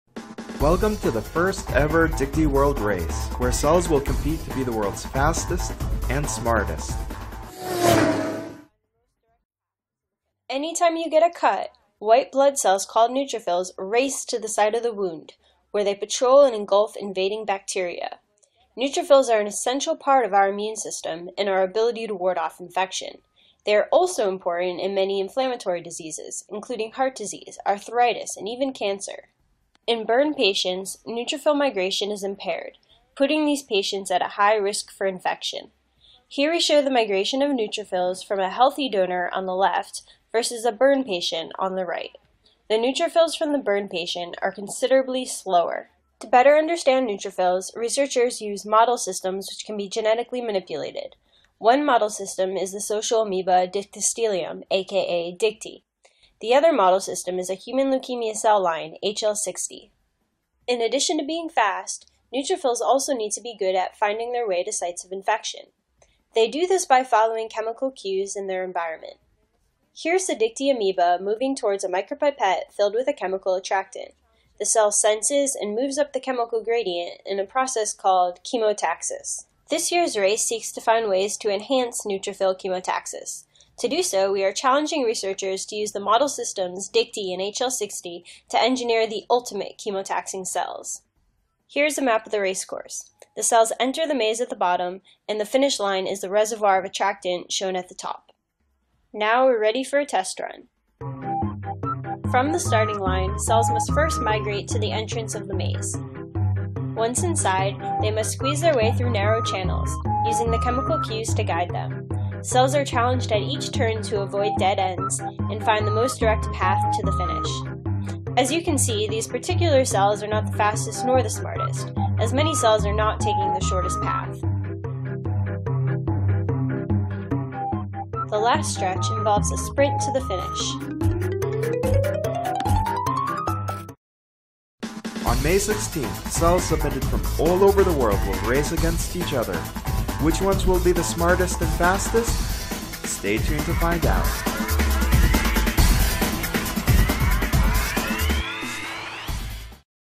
Why the Dicty Race and what is the Dicty Race?
Special thanks to Monica Skoge, PhD and Albert Bae, PhD (UCSD) for making this movie for the Dicty World Race.